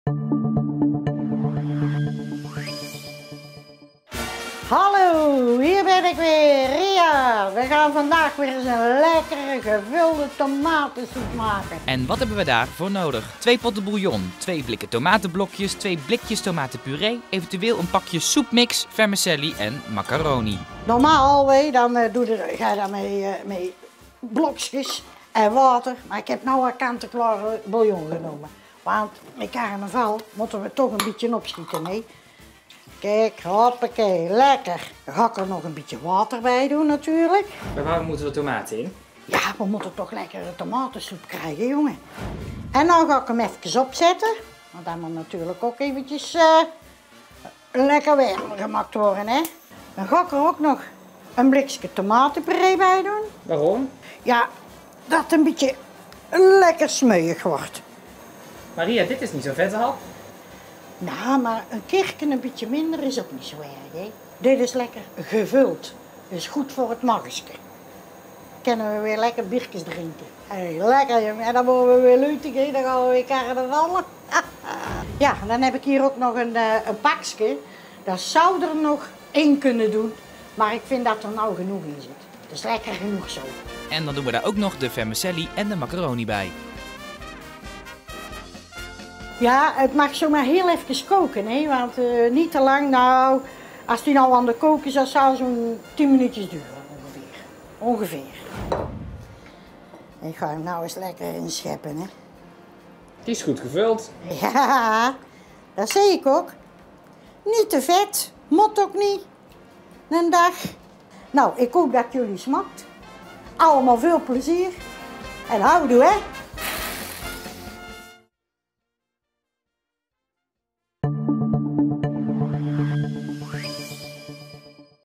Carnavalskok Ria kookt: 'Gevulde tomatensoep, die goed is voor het maguske'
De vette hap staat met Carnaval centraal. En ook onze Carnavalskok Ria houdt van vet eten. Maar zeker nu we op de helft zitten van het Carnaval is het goed om een je maag even rust te geven. Daarom maakt Ria een goed gevulde tomatensoep.